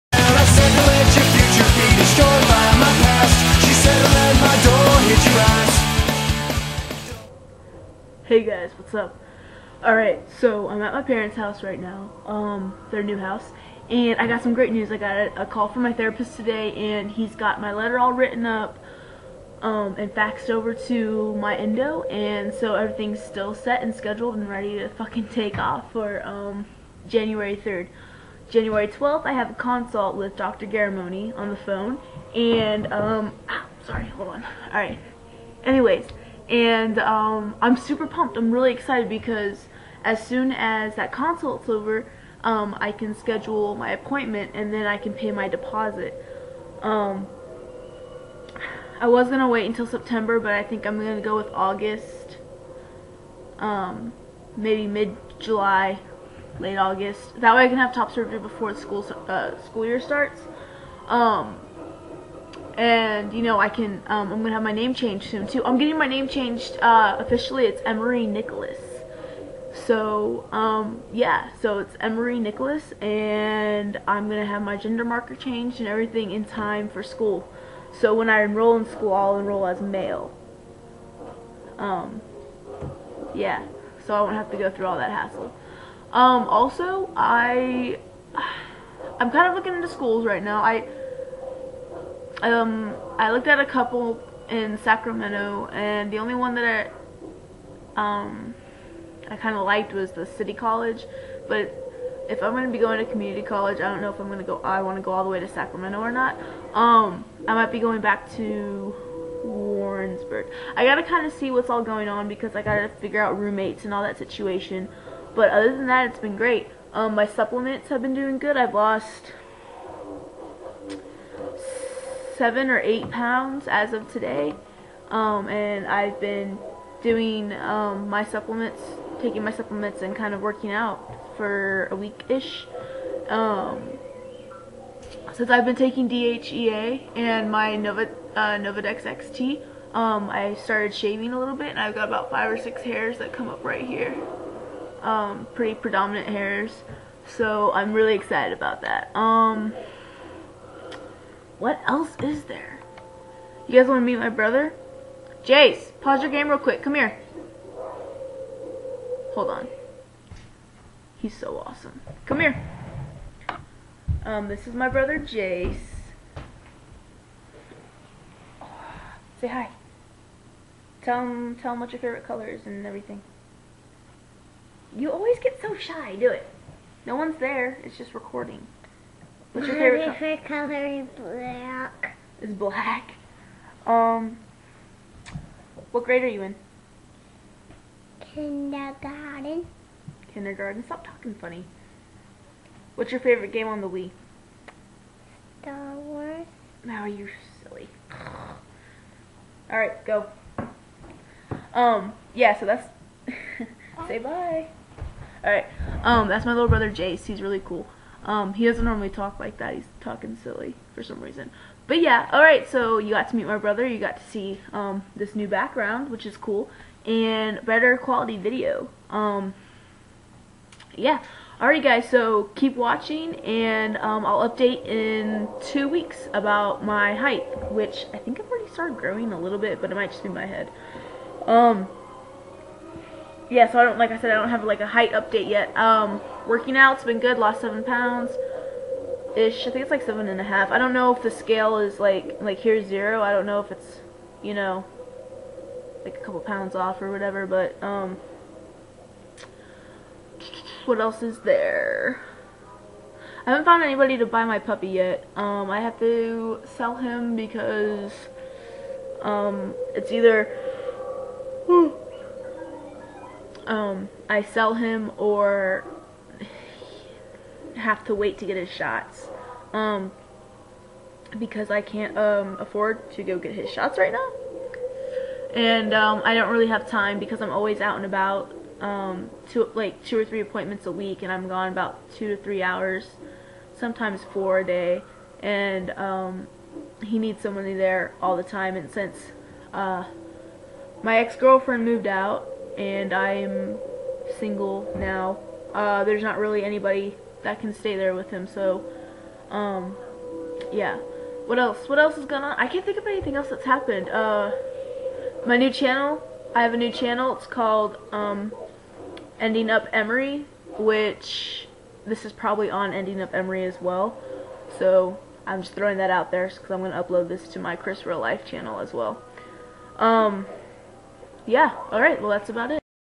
Emery (kevin)FTM Vlog#2 Pre T
Hey guys UPDATES! Starting HRT in 5 days and i have y top surgery consult weight and work out updates MEET MY LITTLE BROTHER JACE!